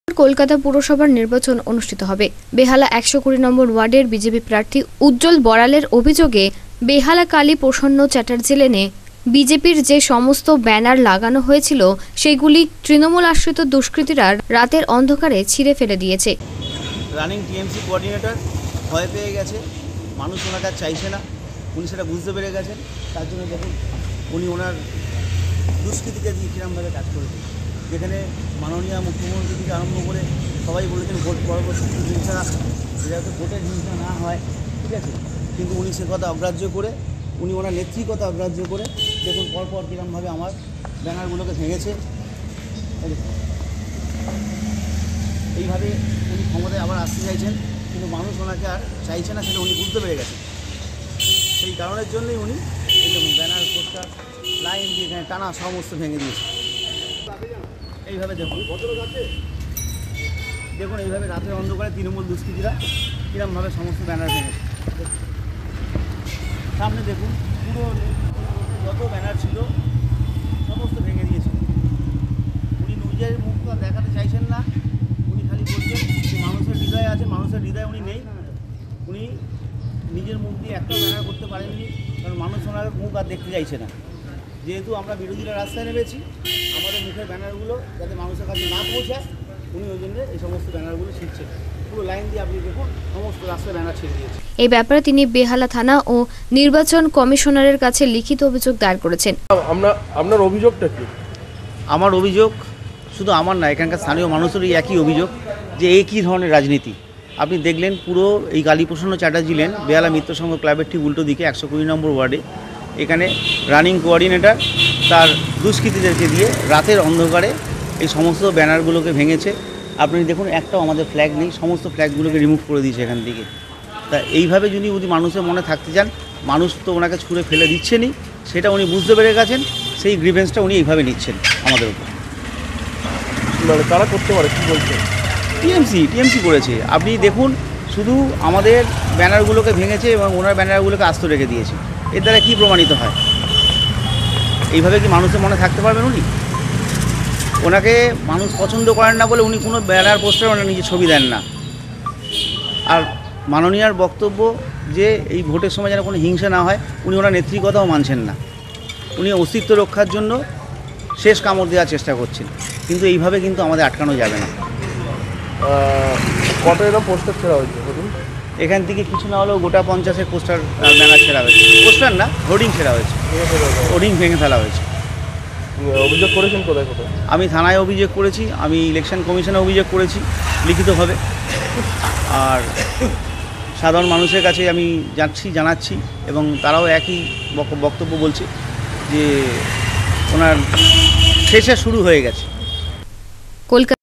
বিজেপি প্রার্থীর অভিযোগ
বিজেপি প্রার্থী উজ্জ্বল বড়াল এর অভিযোগ যে সমস্ত ব্যানার লাগানো হয়েছিল সেইগুলি তৃণমূল আশ্রিত দুষ্কৃতীরা রাতের অন্ধকারে ভেঙে ছিড়ে ফেলে দিয়েছে